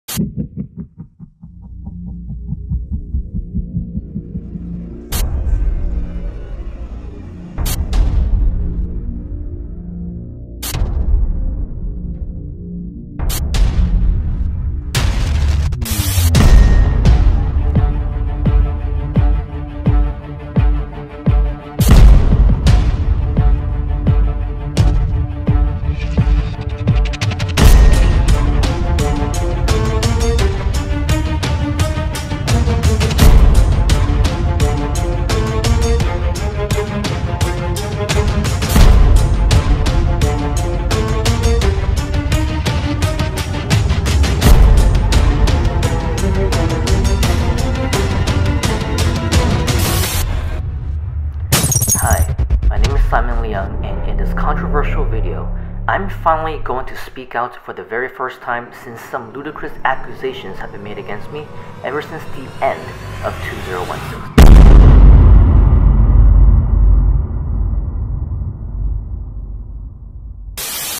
Simon Leung Defamation Case - "The Truth" (Trailer)
Simon Leung has been unlawfully defamed, and to clear up his name, Simon Leung has privately recorded a legal cause of action video that reveals all.

From false accusations to scam allegations, Simon Leung addresses everything that has been accused of, including the rebuttal of all the claims made against him with real evidence.

Unfortunately, for legal reasons, Simon Leung's video cannot be released in its entirety to the public at this time - but for those who are closely associated with this defamation case and are somehow involved, individuals who are directly or indirectly linked may apply to access this video for review purposes.

To submit your expression of interest to be considered to apply for private access to the Simon Leung Defamation Case Video, fill out the appropriate form